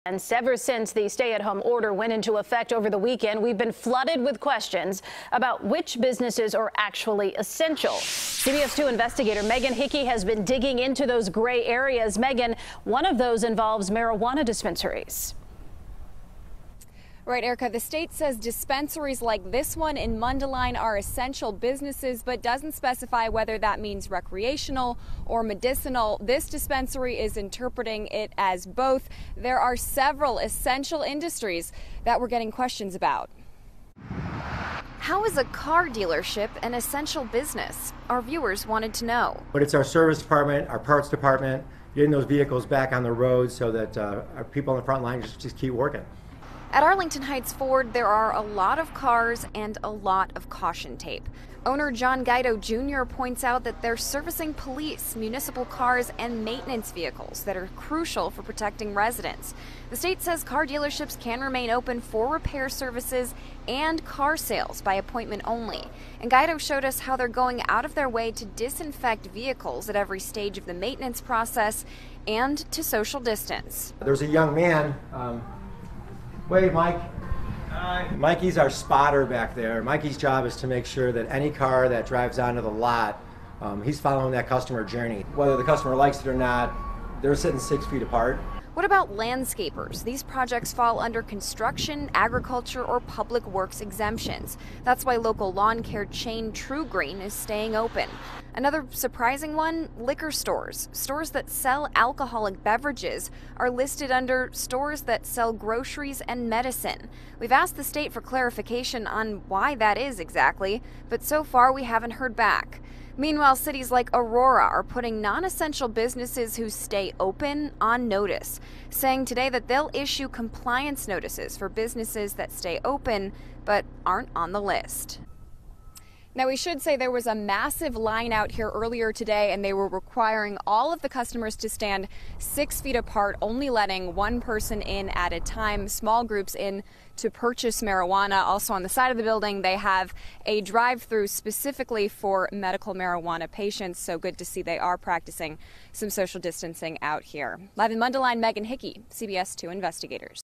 What's Considered An Essential Business During the COVID-19 Pandemic?
CBS 2 Investigator Megan Hickey reports on what is and isn't open during the COVID-19 crisis.